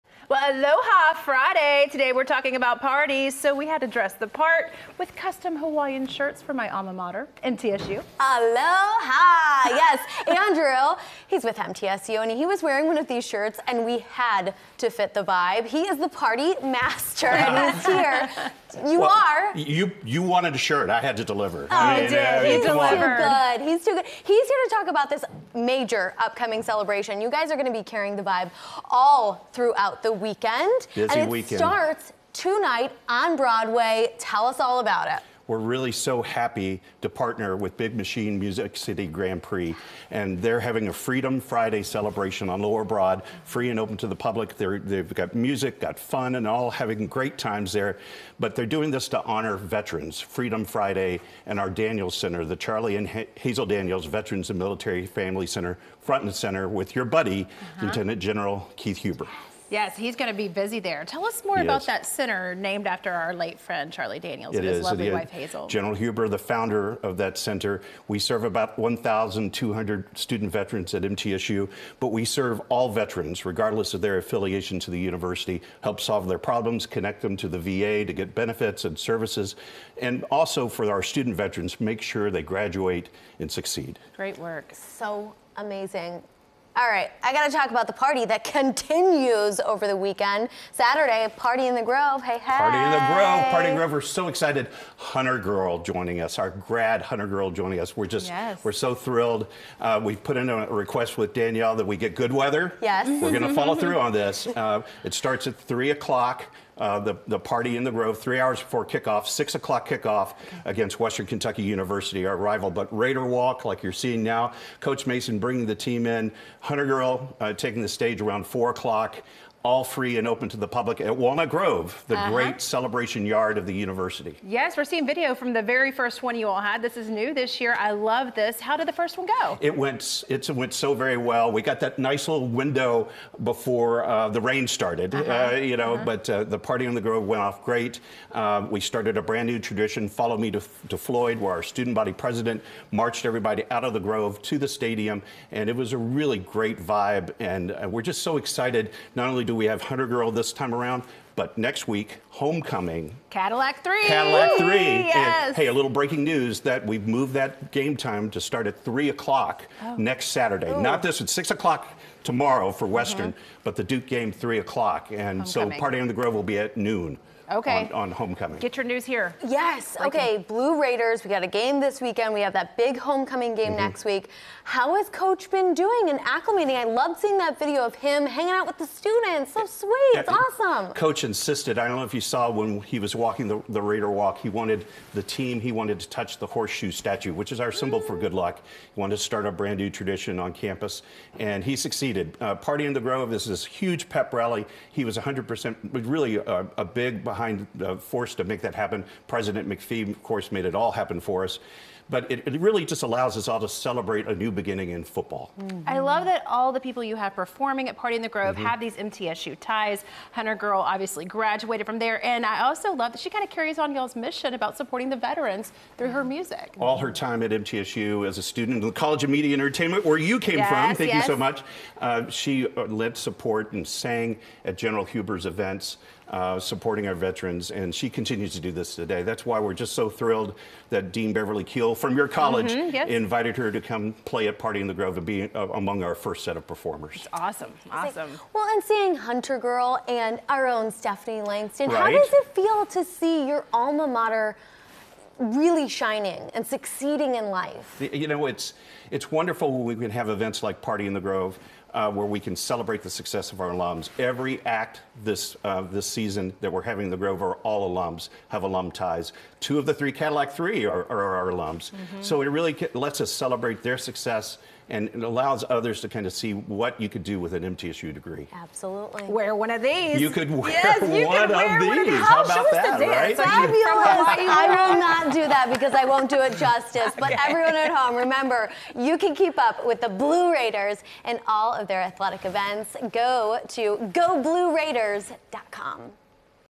MTSU: Go Blue Raiders!
How do you like our new Hawaiian shirts? MTSU is making a fashion statement and showing us how to party this weekend. Rising country star HunterGirl and country-rock trio The Cadillac Three, all with strong Middle Tennessee State University alumni ties, are the next two acts set for Party in the Grove, a new pregame tradition on the Murfreesboro campus.  You will also see MTSU featured at the Big Machine Music City Grand Prix this weekend.  Get hyped! Go Blue Raiders!